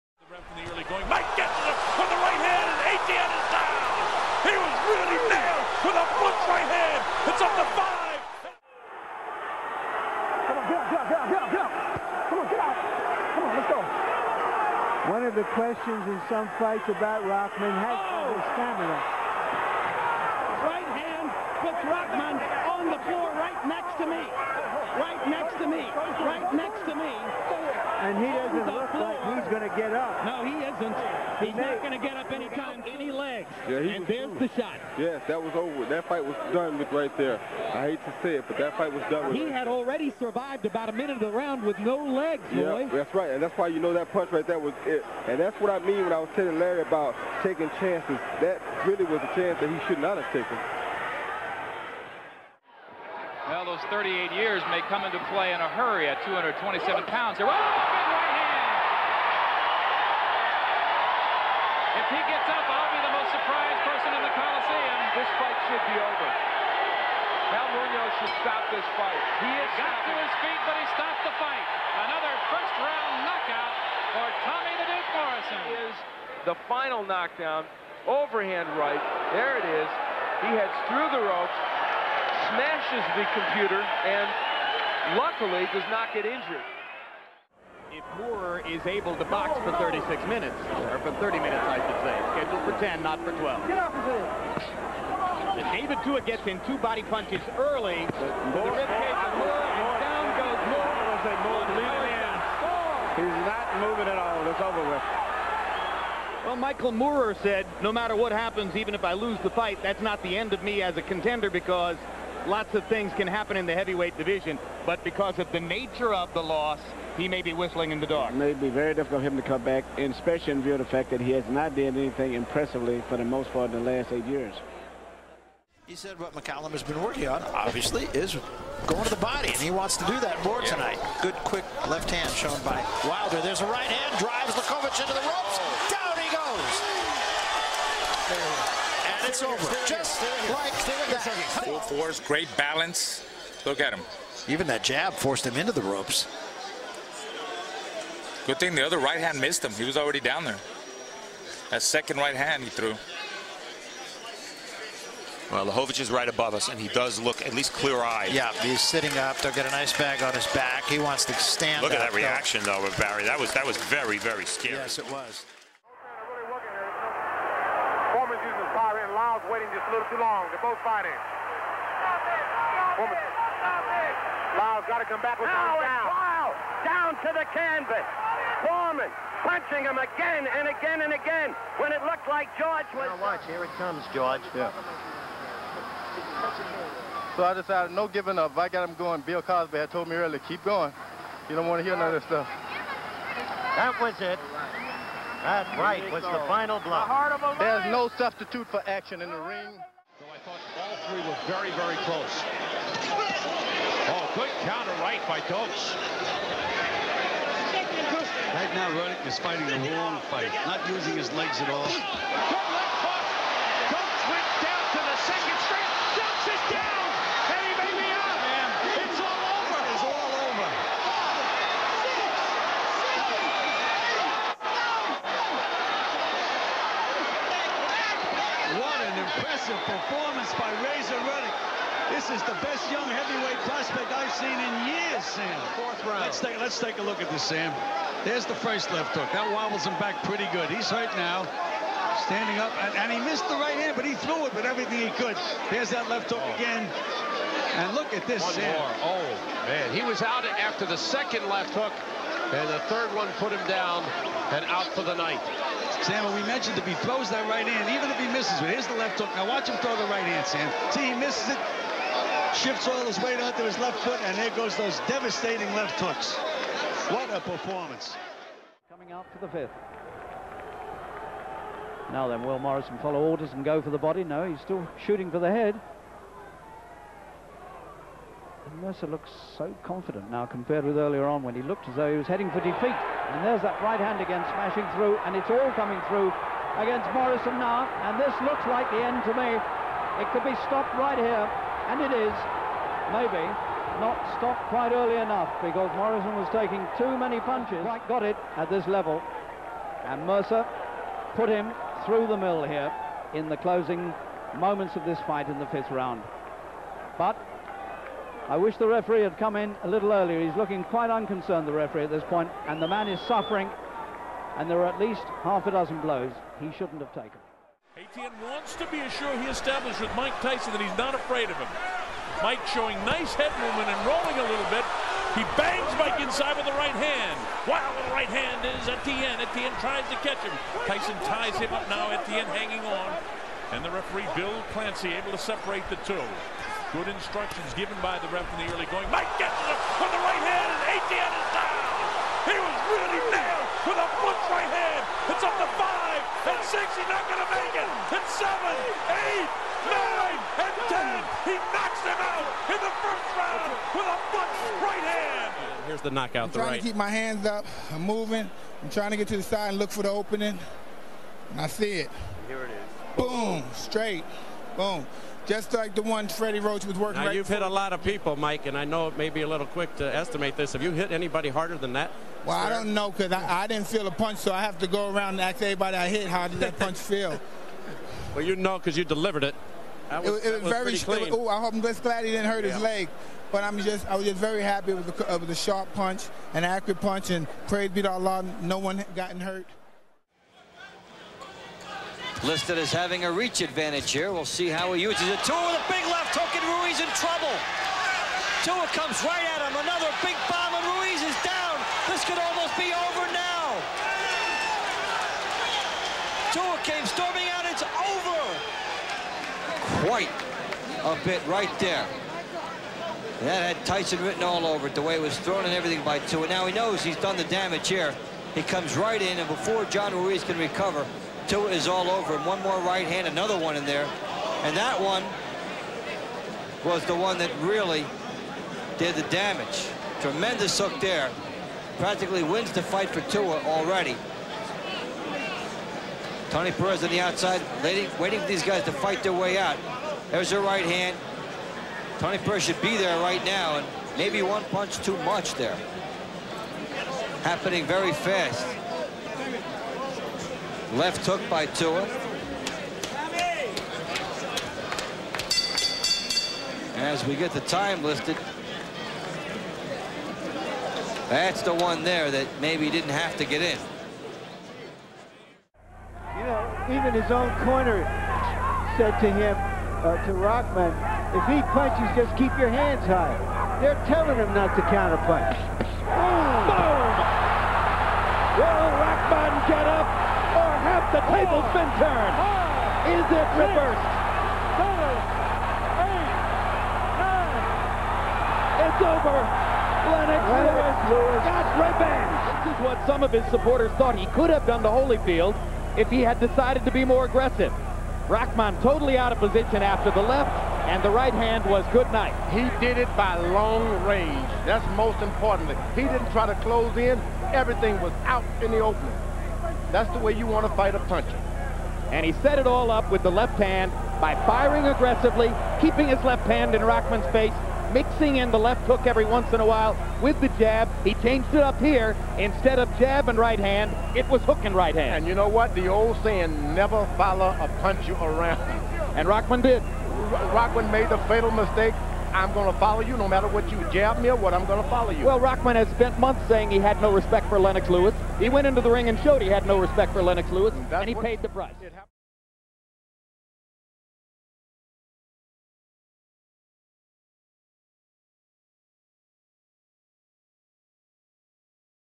BOXING KNOCKOUTS Part 4 - HEAVYWEIGHT SPECIAL
New series highlighting sensational knockouts from boxing's past and present.